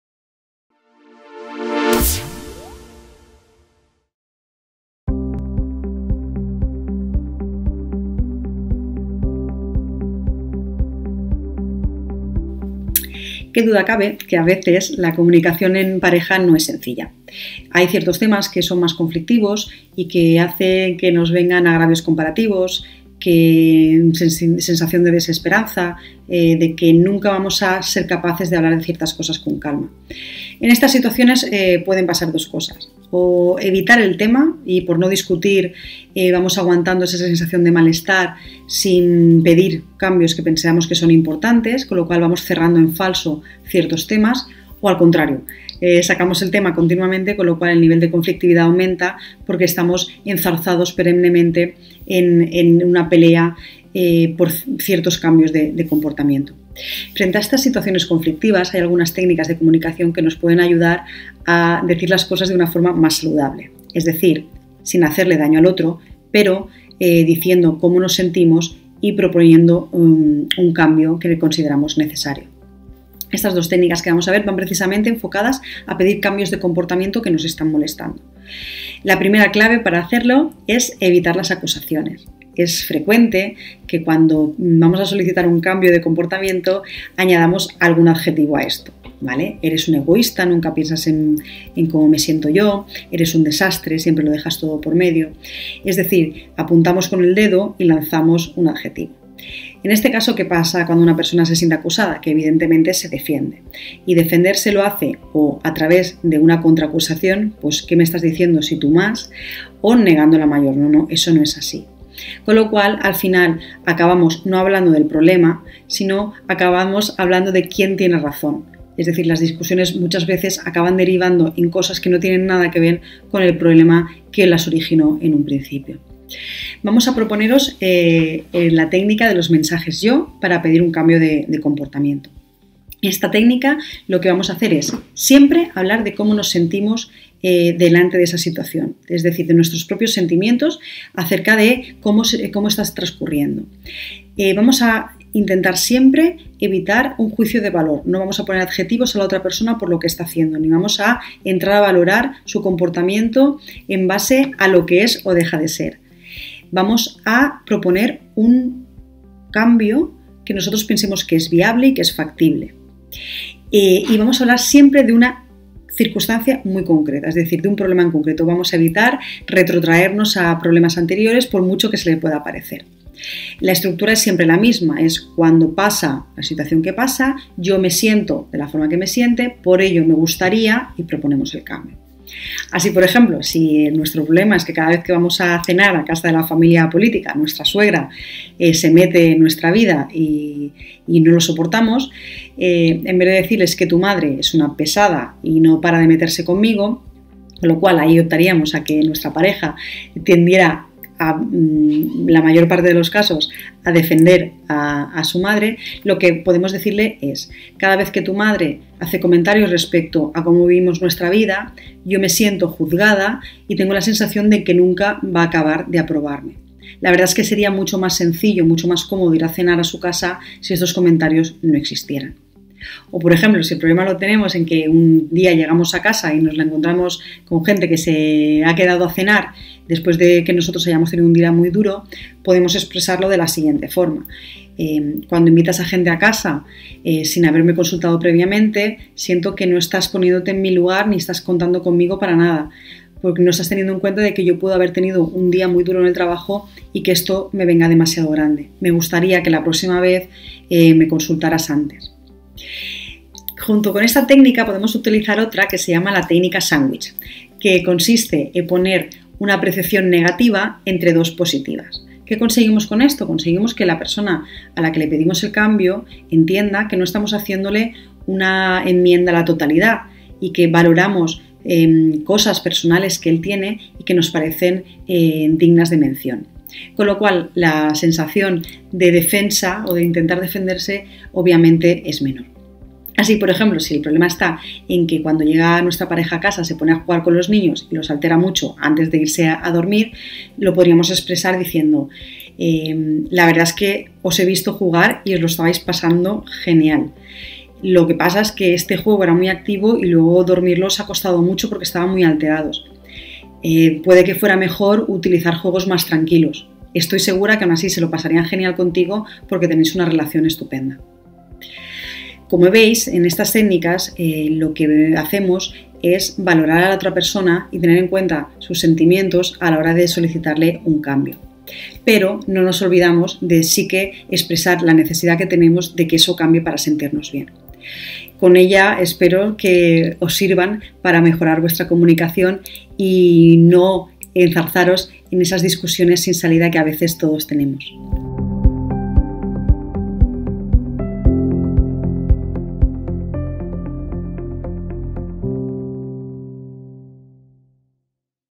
Técnicas para mejorar la comunicación 📢 en la pareja
La comunicación con la pareja no siempre es sencilla. Al hablar de temas conflictivos, podemos aplicar algunas técnicas de comunicación para evitar enzarzarnos en discusiones con la pareja al tratar temas que pueden resultar incómodos.